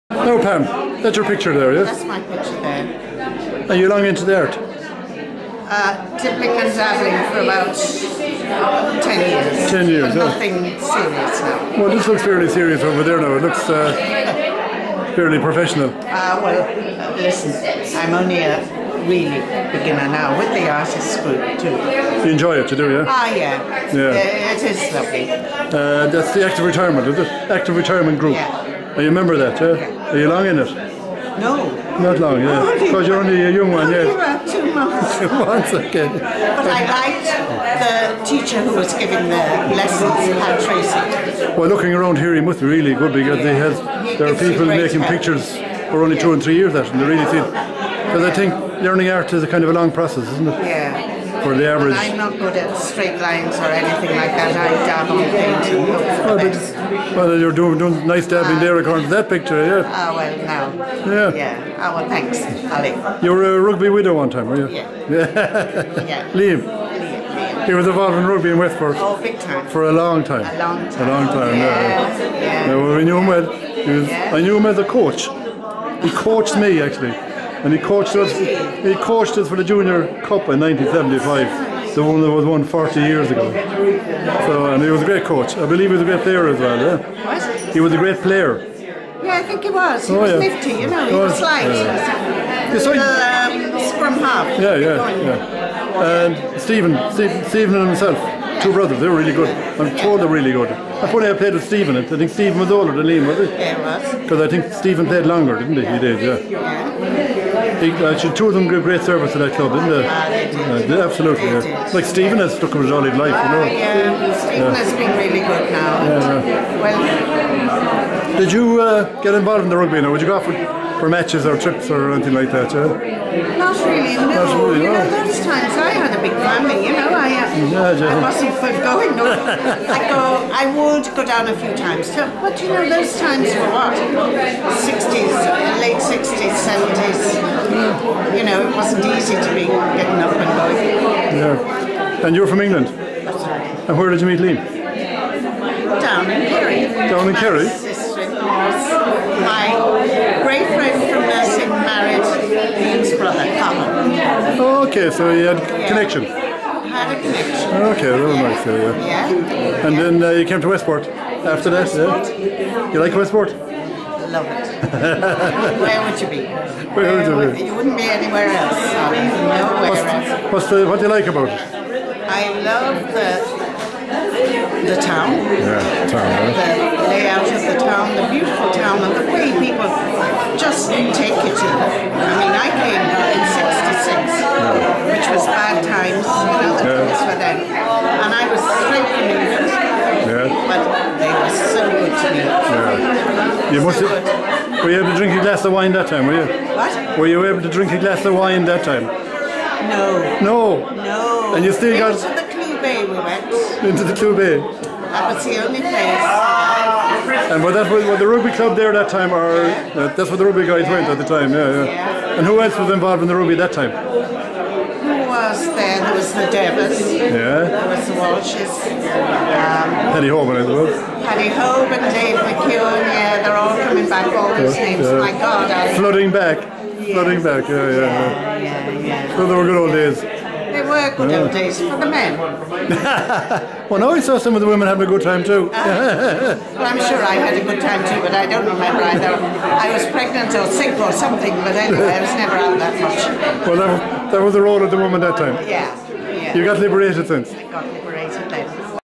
Pam Walsh: Artist
Pam Walsh talks about her art, Westport and rugby in Westport at the opening Of Westport Active Retirement Art Exhibition in Westport Town Hall Theatre. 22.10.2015